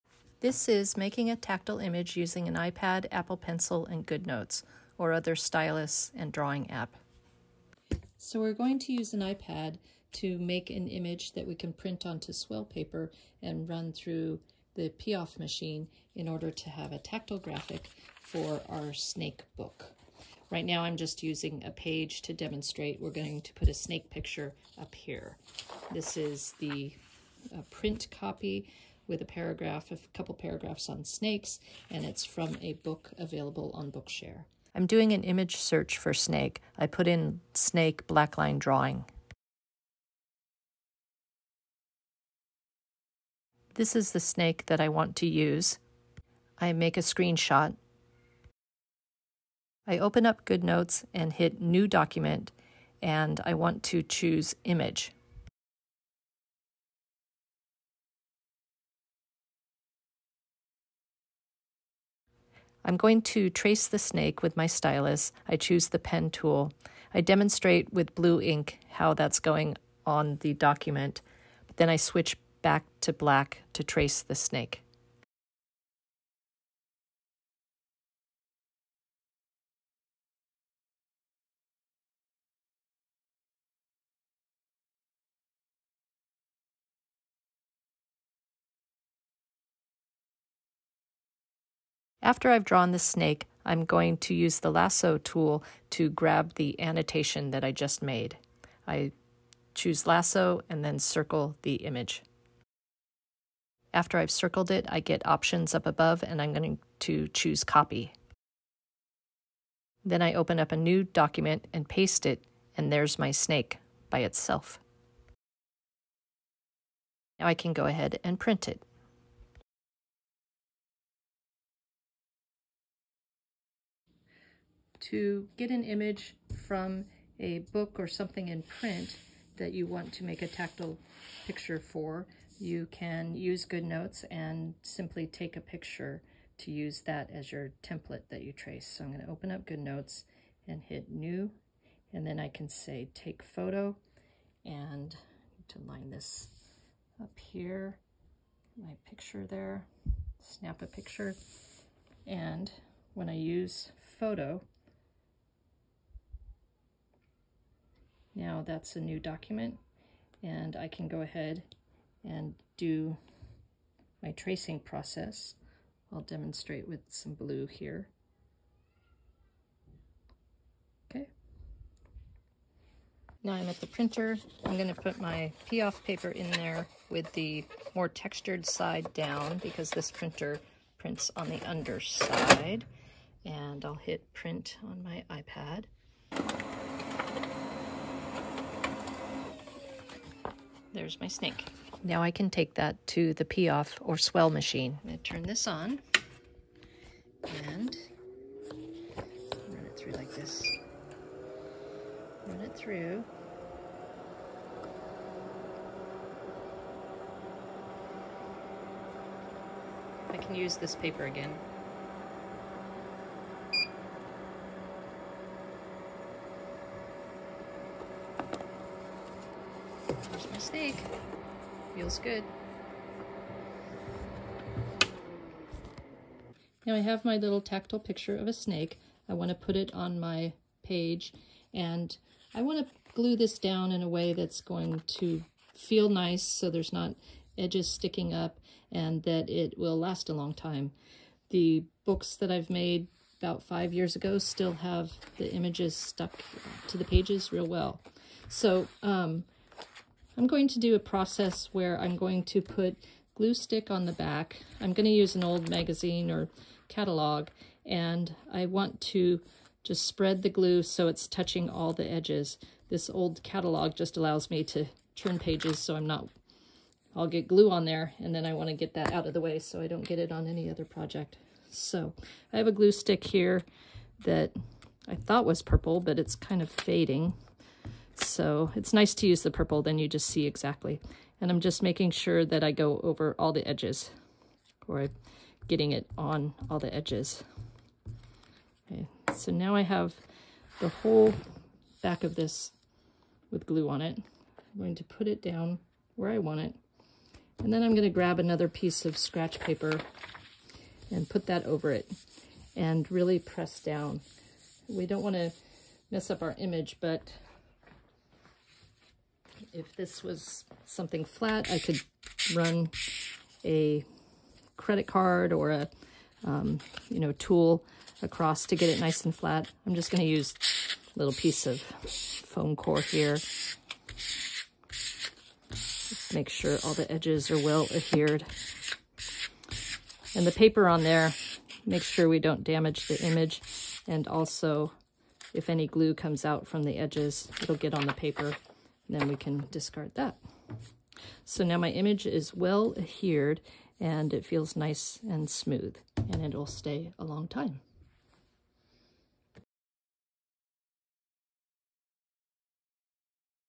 7 HOW TO Make a tactile image using iPad and Goodnotes and Swell Machine
This is making a tactile image using an iPad Apple pencil and good notes or other stylus and drawing app.

1.  Pull up an image from an online search. You can search black line drawings. Or if you have print copy of something you want to make into a tactile image, you can take a picture right from Goodnotes (jump to step 4).
2. Take a screen shot.
3. Open Goodnotes and start a new document with the image option and open your screenshot you just made.
4. Trace the image with your stylus (you are basically annotating the PDF but we will just keep the annotation).
5. Use the lasso tool to copy annotation. Choose lasso and then circle the image. After you've circled it, choose copy. Open up a new Goodnotes document and paste it.
6. Print your drawing onto swell paper
7. Run your swell paper through the PIAF or American Thermoform machine
8. If you are putting the tactile image in a book or in a project, use a glue stick cover the complete back of the image and after you place it down, rub it so it is well adhered (a piece of paper over makes this easy). This way the image feels good on the page without loose edges and the book or project will last a long time.